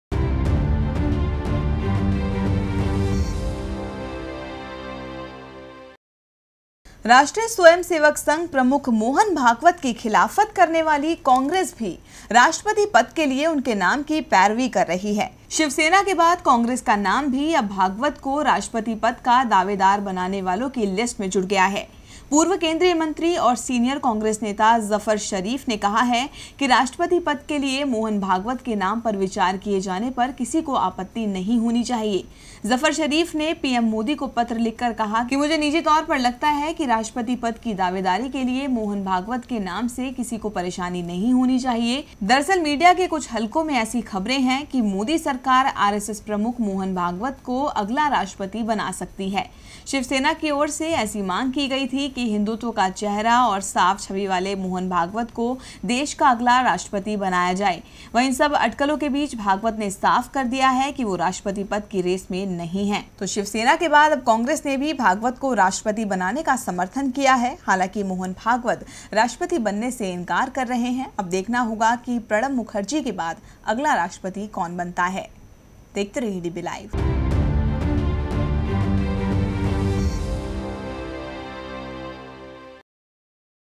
राष्ट्रपति पद के लिए की भागवत के नाम की पैरवी | congress leader backs bhagwat for president
राष्ट्रीय स्वयं सेवक संघ प्रमुख मोहन भागवत की खिलाफत करने वाली कांग्रस भी राष्ट्रपति पद के लिए उनके नाम की पैरवी कर रही है...शिवसेन के बाद अब कांग्रेस का भी नाम भागवत को राष्ट्रपति पद का दावेदार बनाने वालों की लिस्ट में जुड़ गया है..पूर्व केंद्रीय मंत्री और सीनियर कांग्रेस नेता जफर शरीफ ने कहा है कि राष्ट्रपति पद के लिए मोहन भागवत के नाम पर विचार किए जाने पर किसी को आपत्ति नहीं होनी चाहिए...जफर शरीफ ने पीएम मोदी को पत्र लिखकर कहा कि मुझे निजी तौर पर लगता है कि राष्ट्रपति पद की दावेदारी के लिए मोहन भागवत के नाम से किसी को परेशानी नहीं होनी चाहिए...दरअसल मीडिया के कुछ हलकों में ऐसी खबरें हैं कि मोदी सरकार RSS प्रमुख मोहन भागवत को अगला राष्ट्रपति बना सकती है. शिवसेना की ओर से ऐसी मांग की गई थी कि हिदुत्व का चेहरा और साफ छवि वाले मोहन भागवत को देश का अगला राष्ट्रपति बनाया जाए...वहीं इन सब अटकलों के बीच भागवत ने साफ कर दिया है कि वो राष्ट्रपति पद के रेस में नहीं हैं.